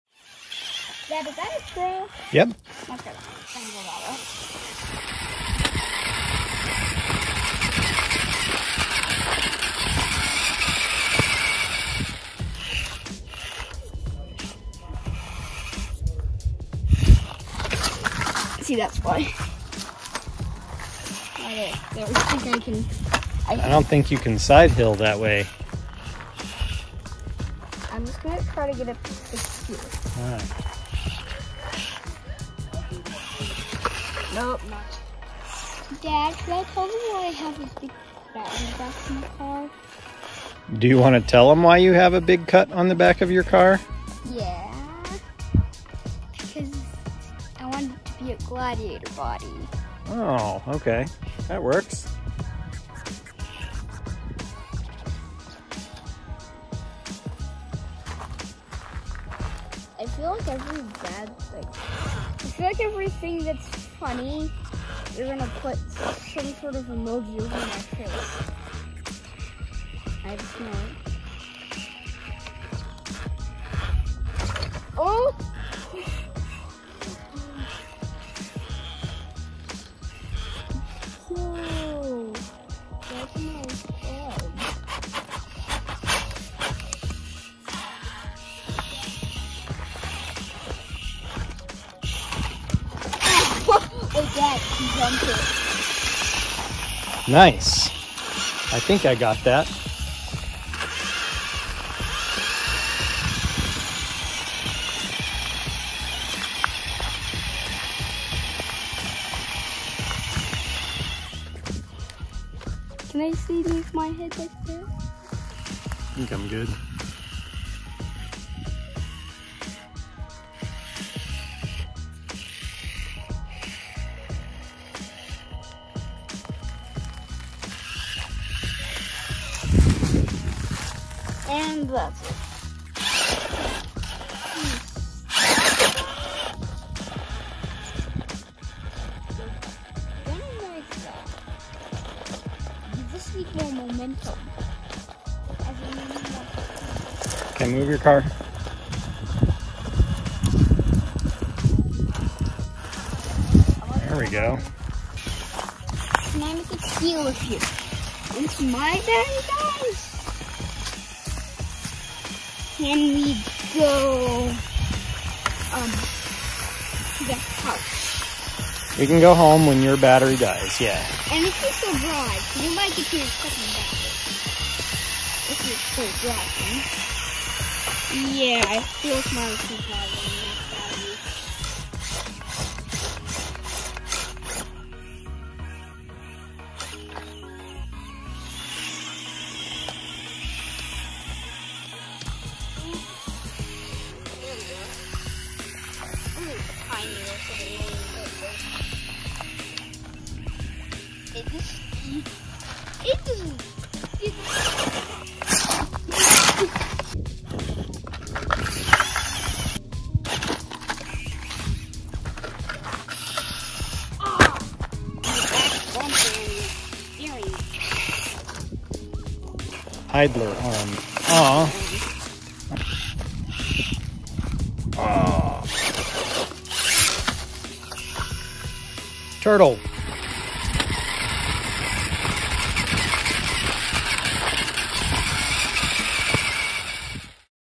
Axial Capra and SCX24 - Rock Crawling w/ Tim!!
I enjoy few things more than spending my time with my kiddos!
Here I'm out taking my car for a walk with one of my sons, who's also taking his car for a walk with me 😁 This is again in Dalla Park, Durango, Co.

Tim wanted his SCX24 Rubicon to look like a Gladiator, so he asked me to cut off the back so it was more pickup shaped. After a while he began anticipating going home and playing video games, and wanted to head home, and was lamenting his battery was lasting so long 😂